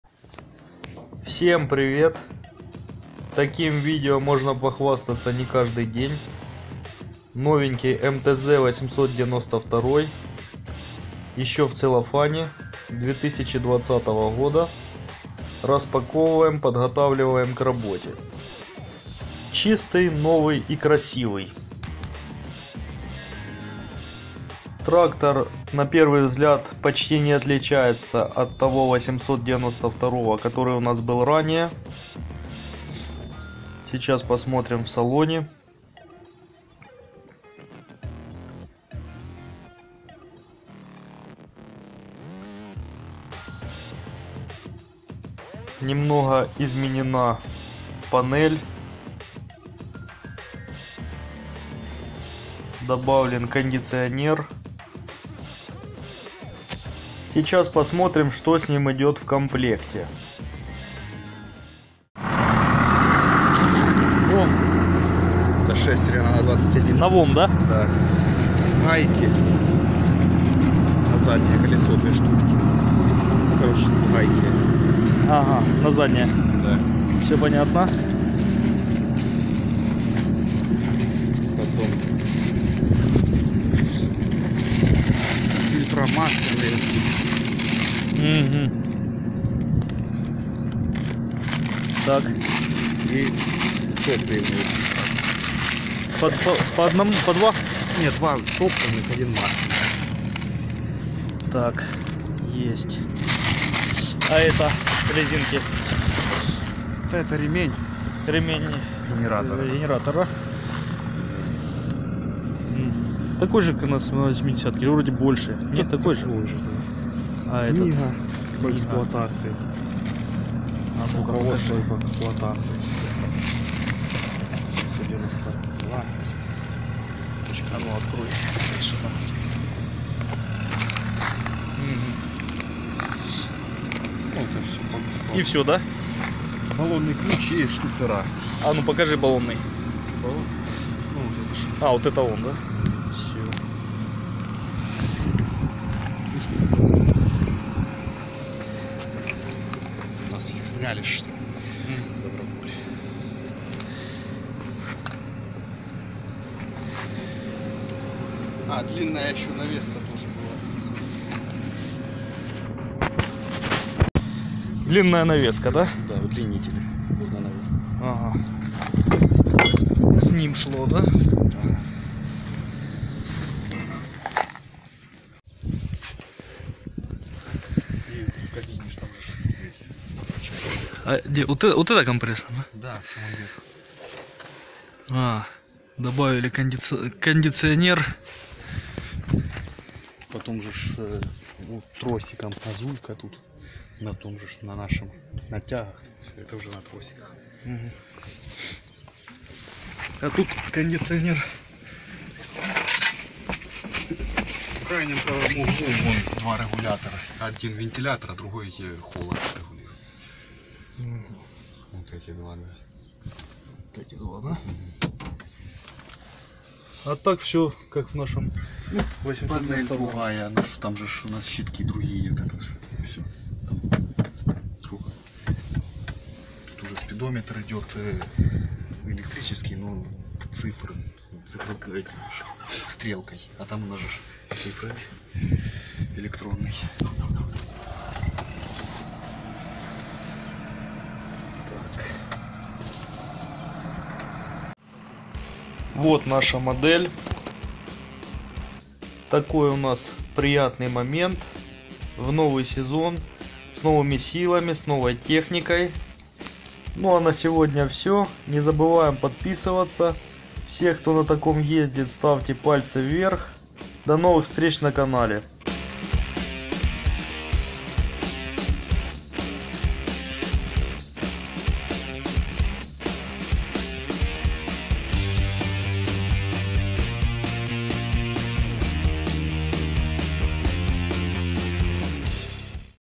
МТЗ 892 выпуск 2020. Обзор, комплектация нового трактора
Новый трактор вводим в работу. Начало сезона с новыми силами и техникой